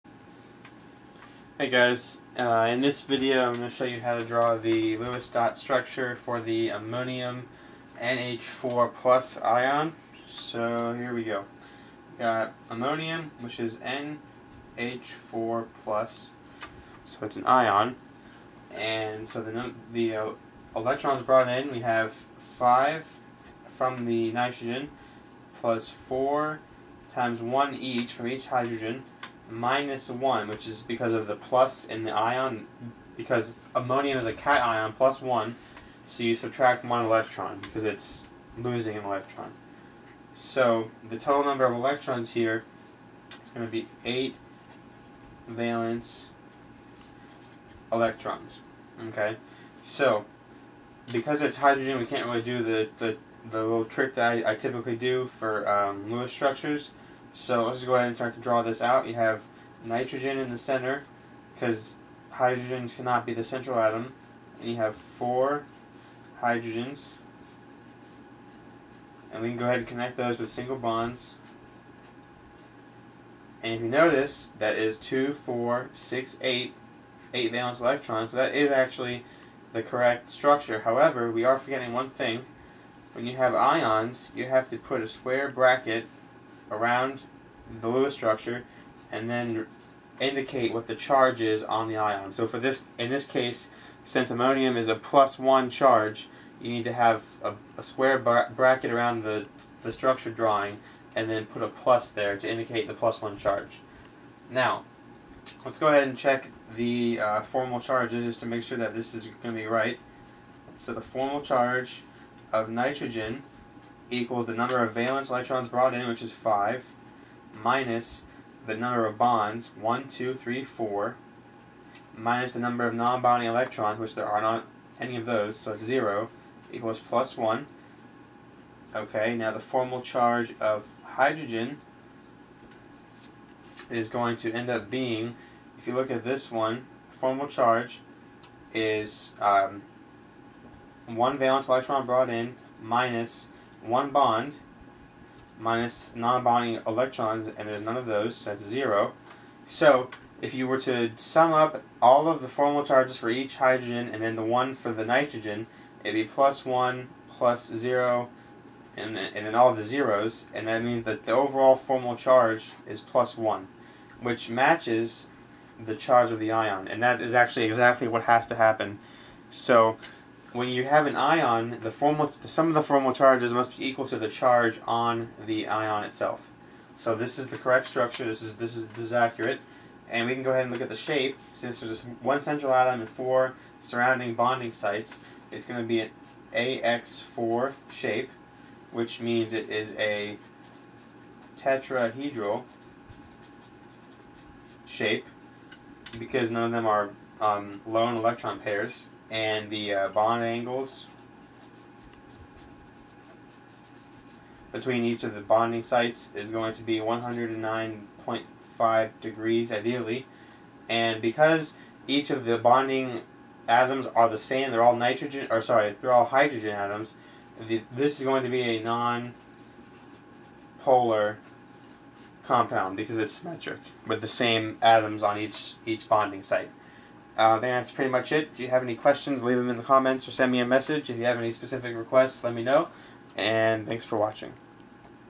Ammonium Ion NH4 + Lewis Dot Structure
A video explanation of how to draw the Lewis Dot Structure for the Ammonium Cation, along with information about the compound including Formal Charges, Polarity, Hybrid Orbitals, Shape, and Bond Angles.